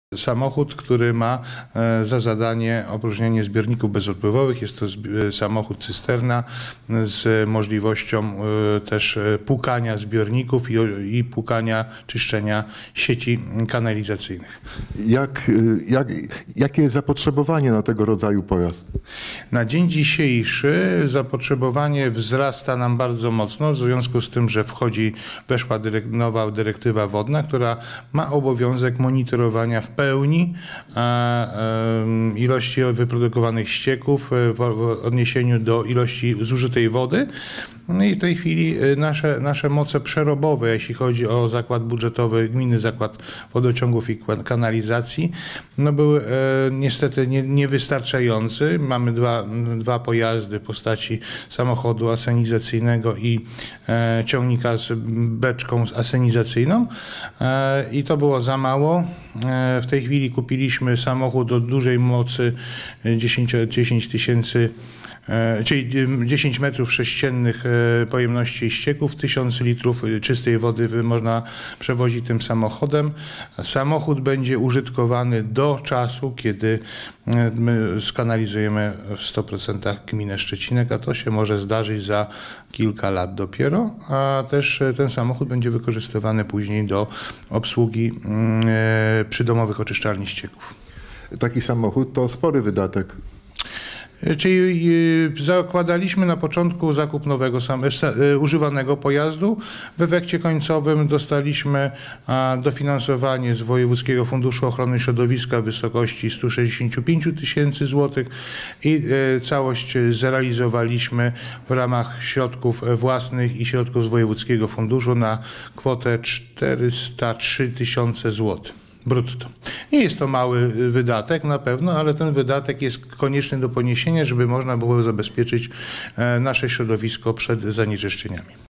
Beczkowóz dla gminy [2017.10.11]
Gmina Szczecinek zakupiła nowy pojazd. Jest fabrycznie nowy samochód do asenizacyjny do wywozu nieczystości z funkcją czyszczenia sieci. Jest to specjalistyczny samochód ze zbiornikiem cylindrycznym o pojemności 10.000 litrów.
 Samochód użytkowany będzie przez Zakład Budżetowy Gminny Zakład Wodociągów i Kanalizacji w Szczecinku i służyć będzie do szybkiego likwidowania awarii wodociągowo - kanalizacyjnych, oczyszczania sieci oraz urządzeń wodociągowo - kanalizacyjnych w tym opróżniania zbiorników, odstojników, studzienek kanalizacyjnych oraz innych celów, gdzie wymagane jest podciśnieniowe opróżnianie zbiorników z nieczystości płynnych i szybki ich transport.
 Wartość zakupionego pojazdu wynosi 403.000 zł brutto. Zakup, jak wiele innych w tym roku uzyskał dofinansowanie w formie dotacji na kwotę 165.000 zł ze środków Wojewódzkiego Funduszu Ochrony Środowiska i Gospodarki Wodnej w Szczecinie.